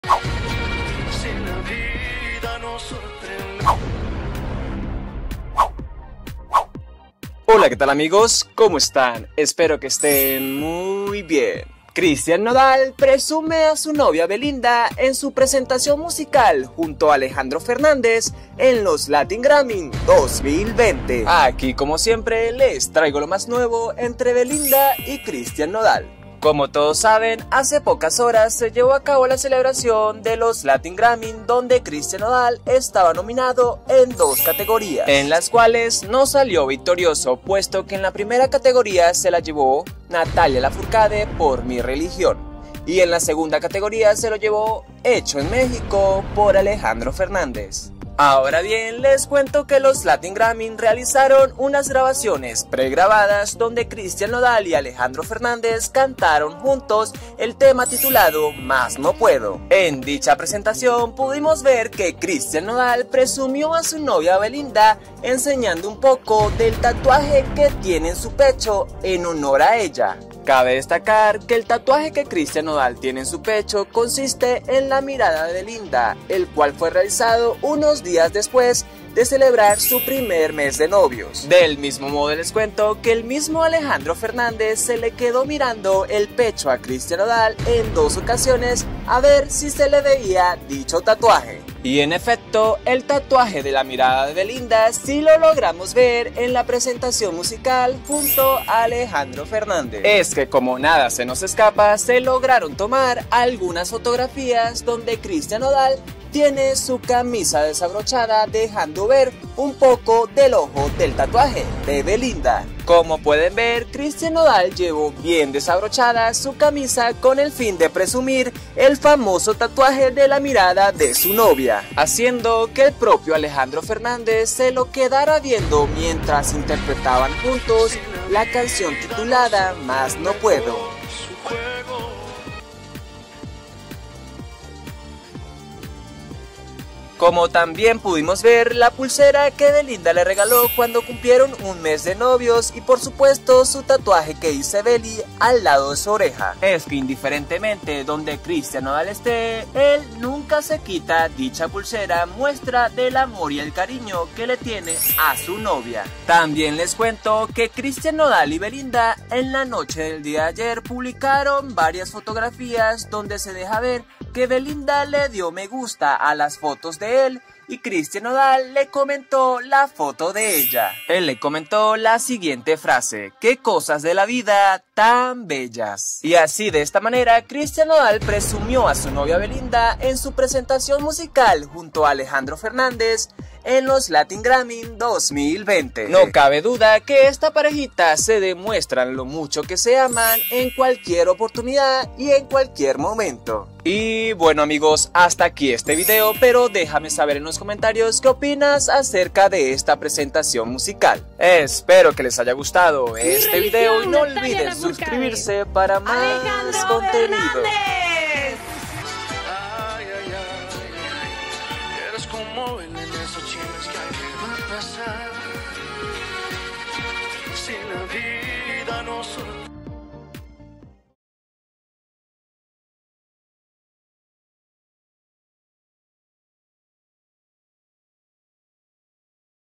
Christian Nodal Presume a su Novia Belinda mientras Canta Con Alejandro Fernández / Maikell Show
CHRISTIAN NODAL PRESUME A SU NOVIA BELINDA MIENTRAS CANTA CON ALEJANDRO FERNÁNDEZ

E-MAIL/CONTACTO/ NEGOCIOS